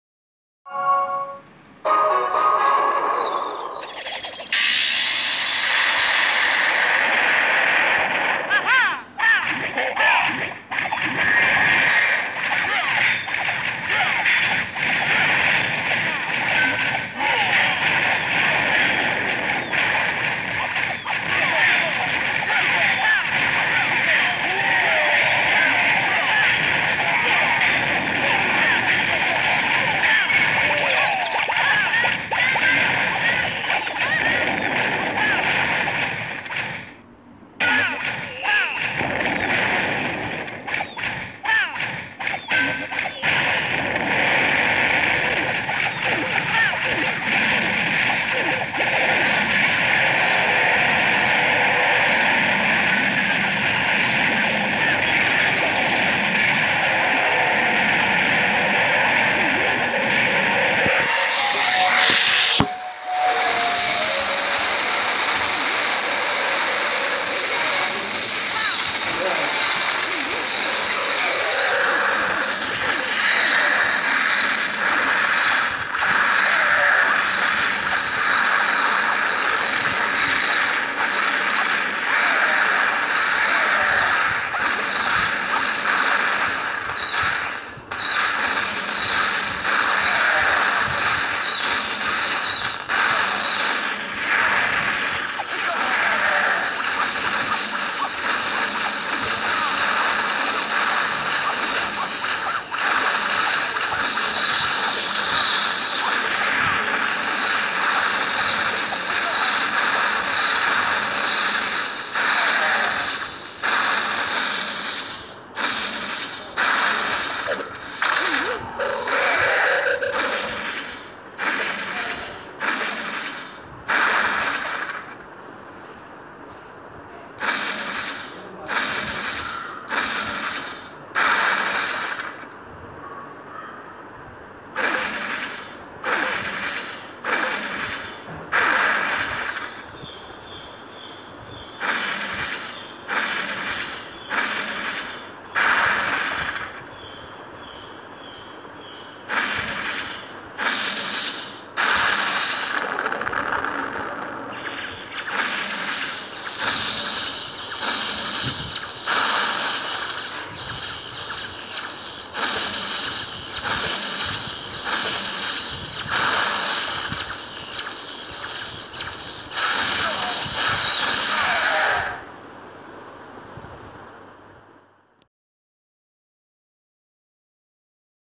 Dragon attack 82% with town hall 10
11 dragons 3 wizards 3 archers spell 4 rages 1 freeze  will do a best attack with these troops mostly target town hall in the side to win easily than town hall middle of the base